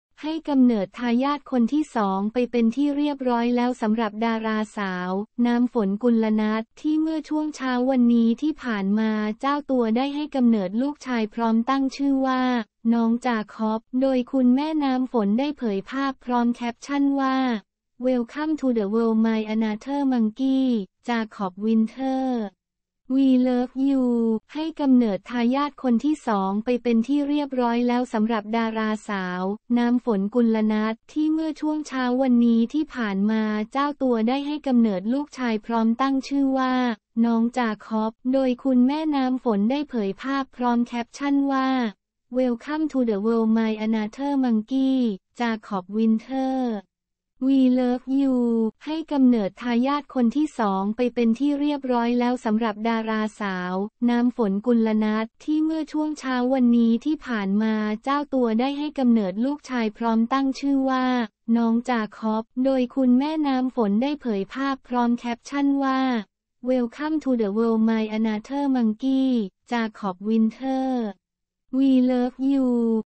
“น้ำฝน กุลณัฐ” คลอดลูกชาย ตั้งชื่อ “น้องจาค็อบ”
ให้กำเนิดทายาท คนที่ 2 ไปเป็นที่เรียบร้อยแล้วสำหรับดาราสาว น้ำฝน กุลณัฐ ที่เมื่อช่วงเช้าวันนี้ที่ผ่านมาเจ้าตัวได้ให้กำเนิดลูกชายพร้อมตั้งชื่อว่า น้องจาค็อบ โดยคุณแม่น้ำฝนได้เผยภาพพร้อมแคปชั่นว่า "Welcome to the world my another monkey.. Jacob Winter. We love you"